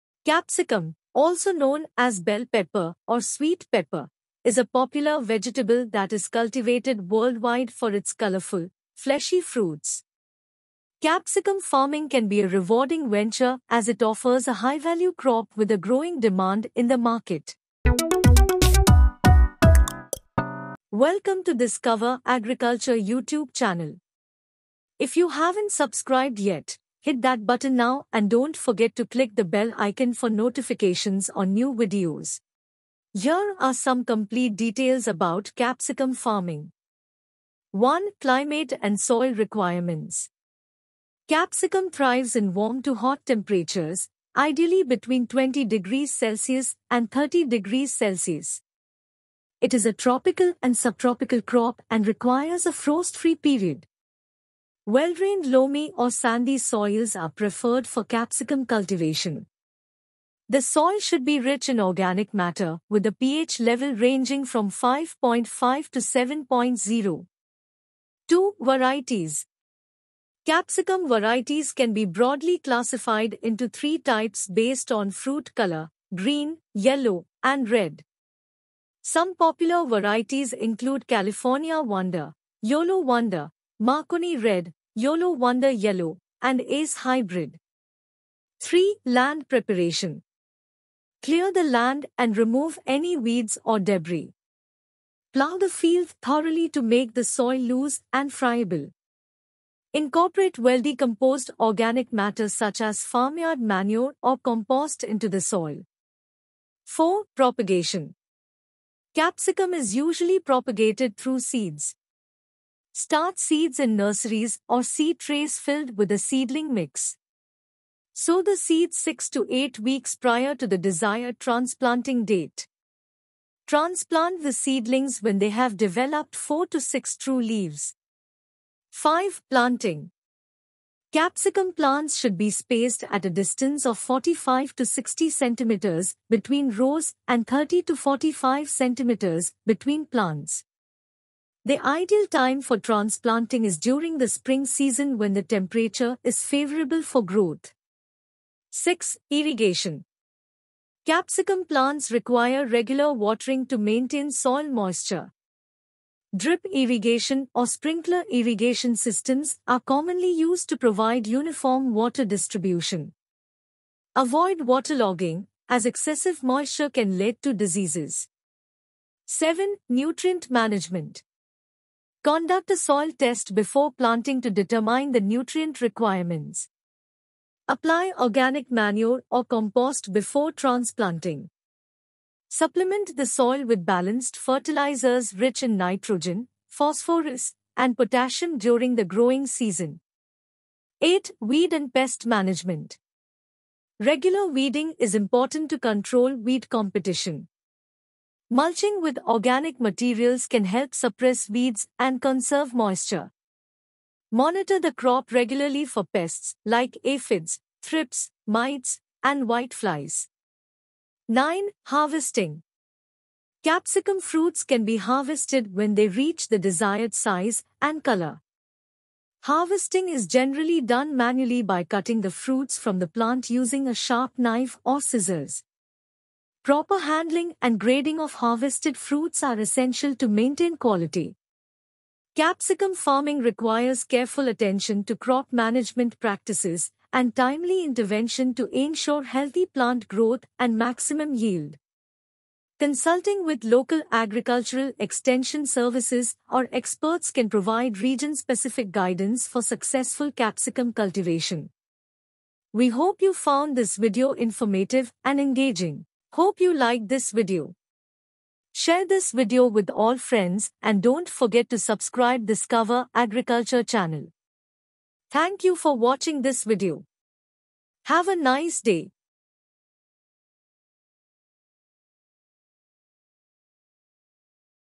Capsicum (Bell Pepper) Farming | How to grow Capsicum from seed at Home | Bell Pepper Cultivation
Welcome to our comprehensive guide on Capsicum (Bell Pepper) farming! In this video, we will provide you with step-by-step instructions on how to grow Capsicum (Bell Pepper) from seed right in the comfort of your own home. Whether you're a beginner or an experienced gardener, this tutorial will equip you with valuable insights and techniques to successfully cultivate this versatile and nutritious vegetable.

Capsicum, also known as Bell Pepper, is a popular vegetable known for its vibrant colors, crunchy texture, and rich flavor. By starting from seeds, you have the advantage of controlling the entire growing process and ensuring the highest quality products. Our expert horticulturist will walk you through the entire journey, from selecting high-quality seeds to nurturing your plants until harvest.

In this video, you'll learn about the key steps and considerations for successful Capsicum cultivation. We will cover essential topics such as soil preparation, seed germination techniques, transplanting seedlings, watering and fertilization requirements, pest and disease management, and more.

Growing Capsicum from seed at home offers numerous benefits, including the joy of witnessing the entire growth cycle, cost savings, and the satisfaction of enjoying homegrown, flavorful Capsicum. By following our practical tips and tricks, you'll be able to maximize your yields and ensure healthy, thriving plants.

Key topics covered in this video:
0:00 Capsicum Farming
0:17 Introduction
0:38 Climate & Soil Requirements
1:08 Varieties
1:29 Land Preparation
1:47 Propagation
2:09 Planting
2:29 Irrigation
2:50 Nutrient Management
3:11 Weed and Pest Management
3:31 Harvesting
3:52 Conclusion
4:16 Bottom line

Join us in this educational and informative tutorial, and learn how to grow Capsicum (Bell Pepper) from seed at home. Let's grow vibrant and flavorful Capsicum (Bell Pepper) together!